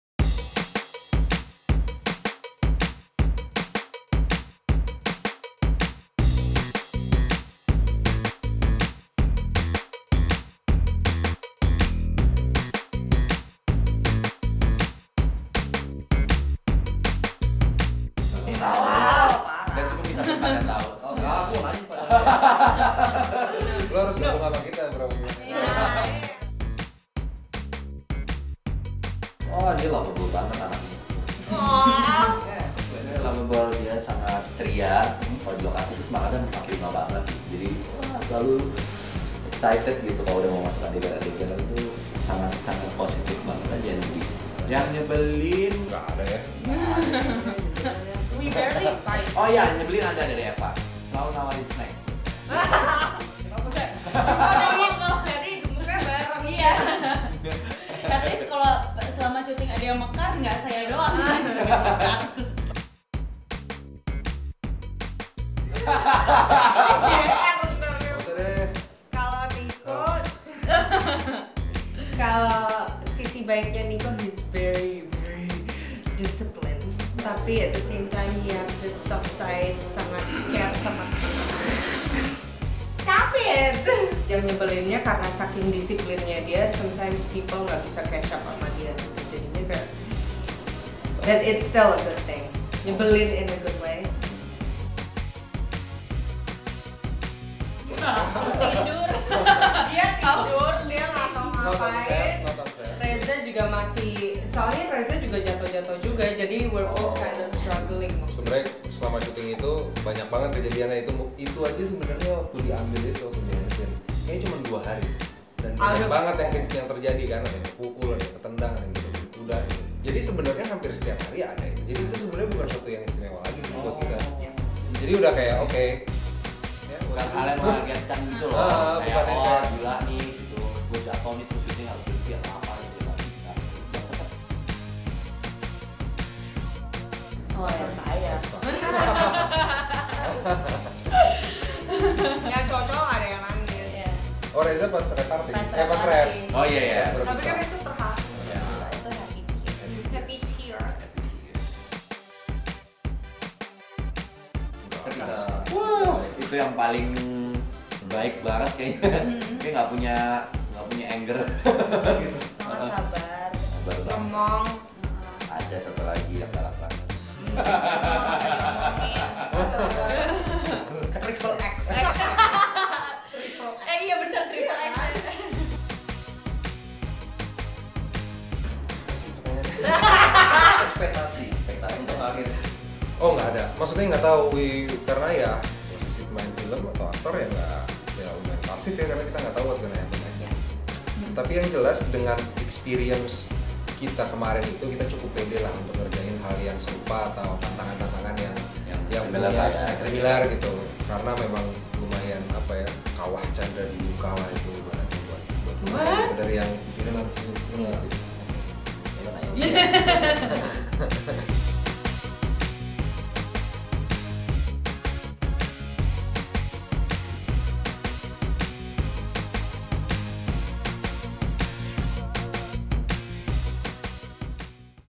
EXCLUSIVE: Ketika Para Pendekar Tongkat Emas Turun Gunung
Nicholas Saputra, Reza Rahadian, Eva Celia dan Tara Basro sambil tertawa-tawa membongkar rahasia tentang banyak hal dalam syuting film Pendekar Tongkat Emas. Mulai dari siapa yang paling sering nangis, orang terrgalak, sampai kejelekan masing-masing. Tonton video wawancaranya eksklusifnya di sini.

Untuk berbagai berita & informasi seputar Film, TV, Selebriti & Entertainment lainnya,